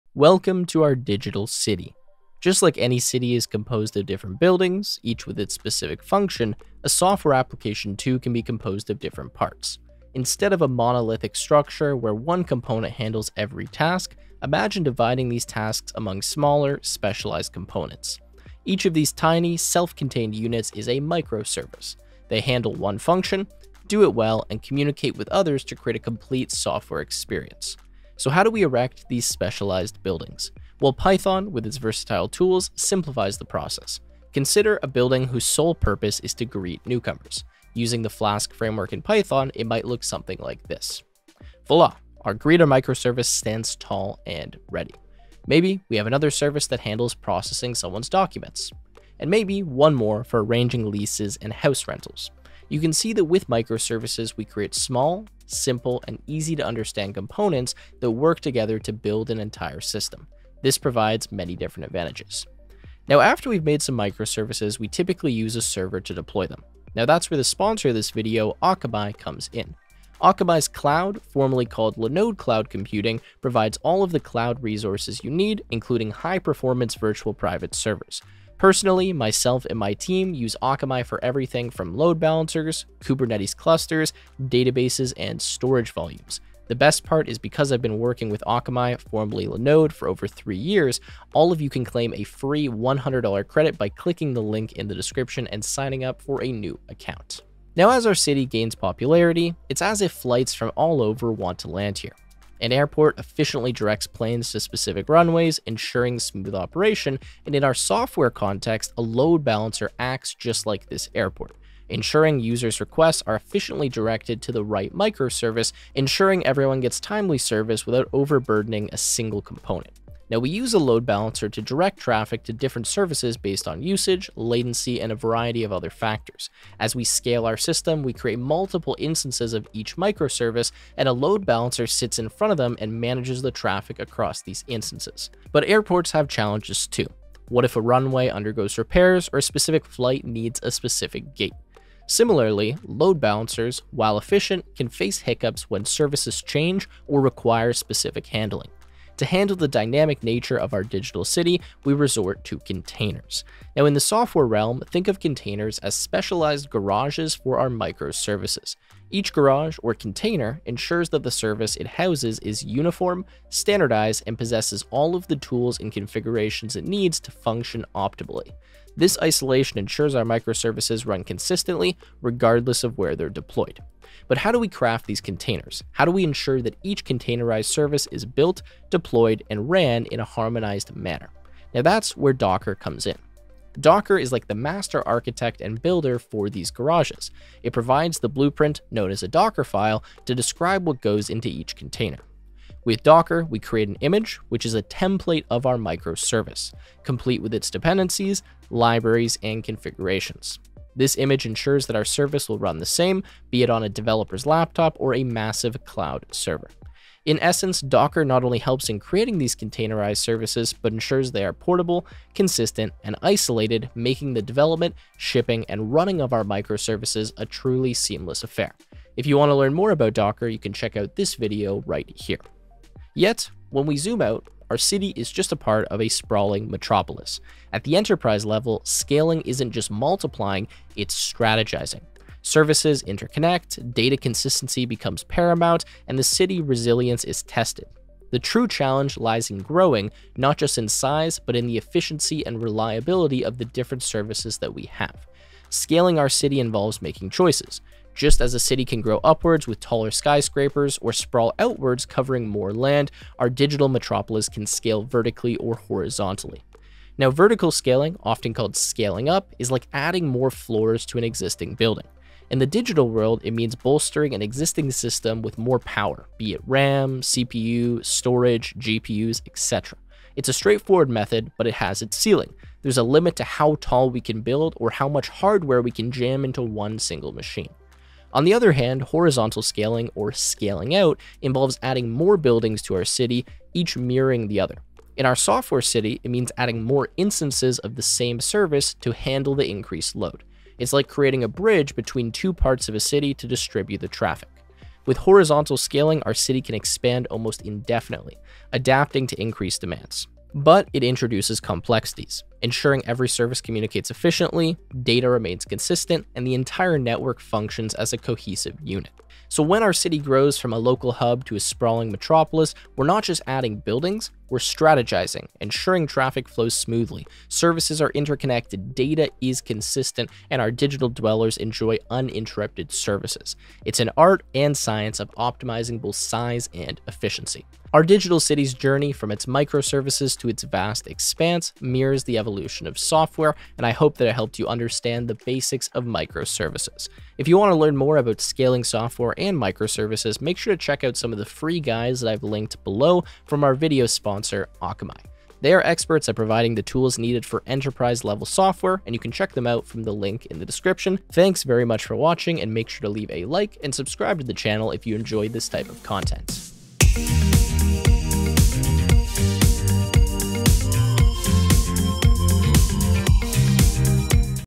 The Magic of Microservices
Today, I'm going to teach you about Microservices! Instead of one component handling every single task, you can divide these tasks across smaller, specialized components. Each of these tiny, self-contained units are microservices that communicate with others to create a complete software experience.

⭐️ Tags ⭐️
- Tech with Tim
- Micro Services
- Programming